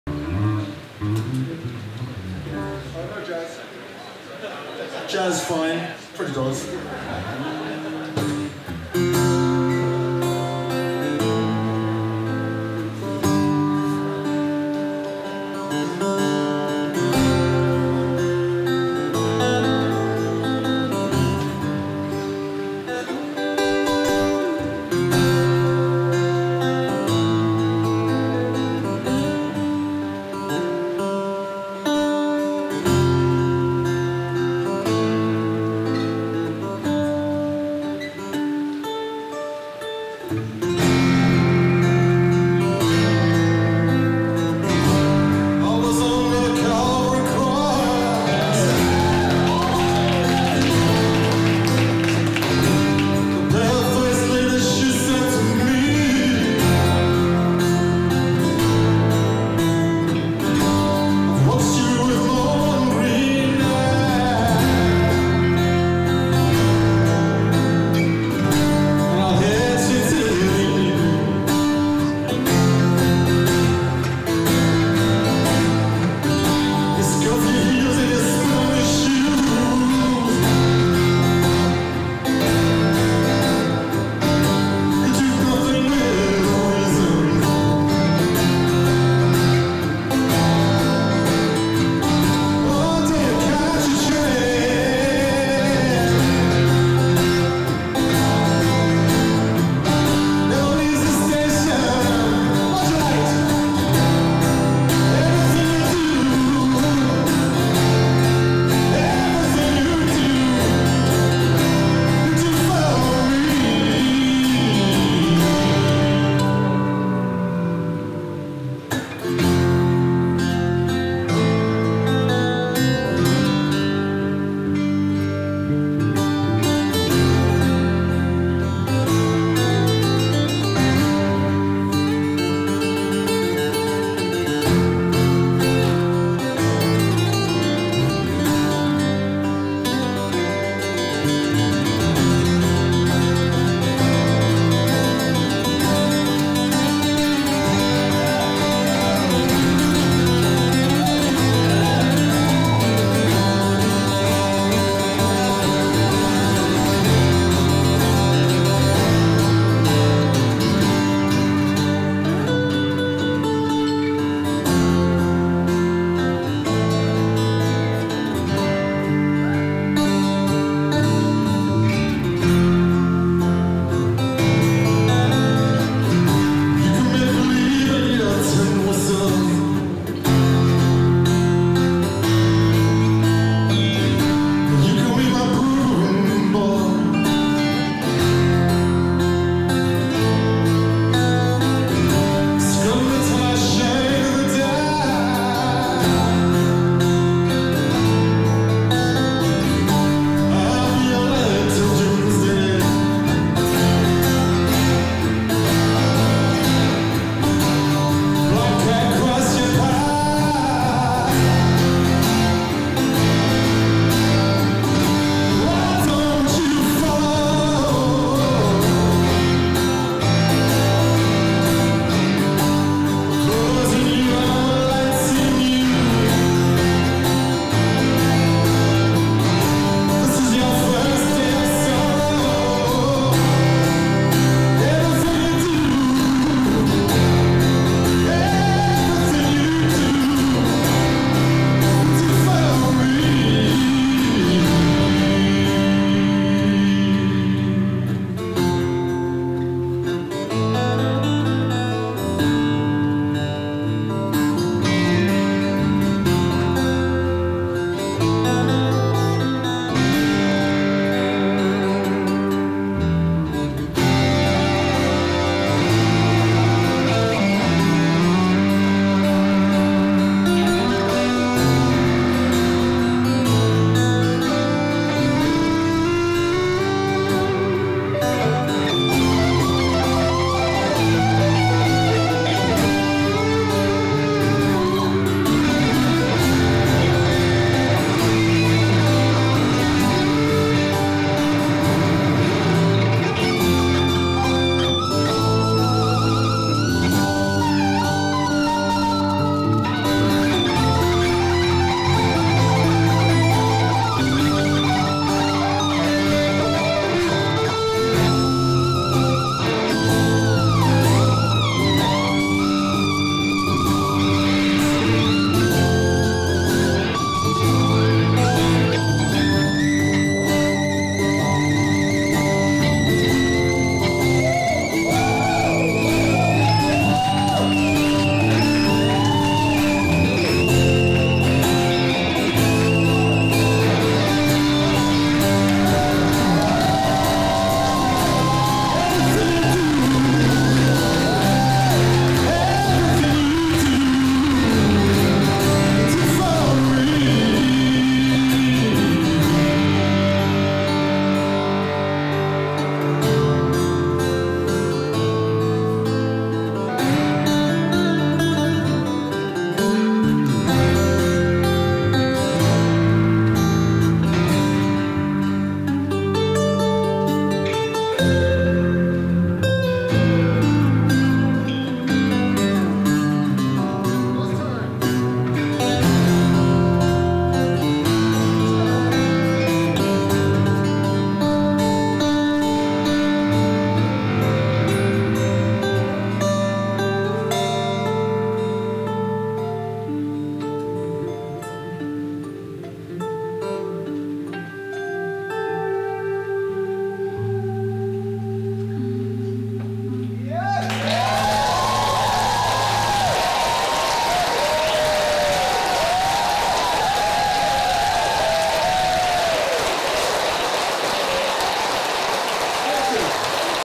Henry Kaiser KILLS it with Richard Thompson
Richard Thompson at Frets and Refrains featuring Henry Kaiser, Zack Hobbs and Jack Thompson.